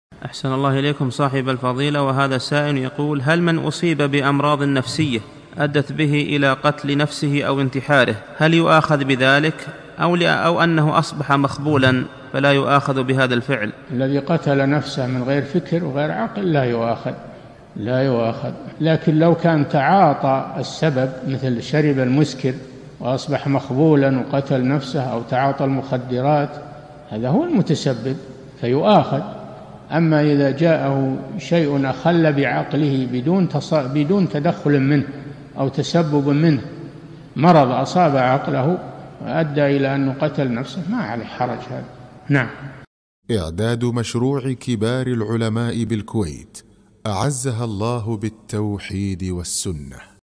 [240 -560] هل يؤاخذ من ابتلي بمرض نفسي فقتل نفسه؟ - الشيخ صالح الفوزان
-"السؤال: هل من أصيب بأمراض نفسية أدت به إلى قتل نفسه أو انتحاره، هل يؤاخذ بذلك، أو أنه أصبح مخبولاً فلا يؤاخذ بهذا الفعل؟
الجواب: الذي قتل نفسه من غير فكر وغير عقل لا يؤاخذ، لا يؤاخذ، لكن لو كان تعاطى السبب مثل شَربَ المسكر وأصبح مخبولا وقتل نفسه أو تعاطى المخدرات هذا هو المتسبب فيؤاخذ، أما إذا جاءه شيء أخل بعقله بدون تدخل منه أو تَسبُبٍ منه، كمرض أصاب عقله وأدى إلى أنه قتل نفسه ما عليه حرج هذا.
للاشتراك في قناة الشيخ صالح الفوزان
سؤال وجواب من شرح السنة للبربهاري ( 560 ) مقطع

للاستفسار والإقتراحات والدعم
رئيس الفريق
لمشروع كبار العلماء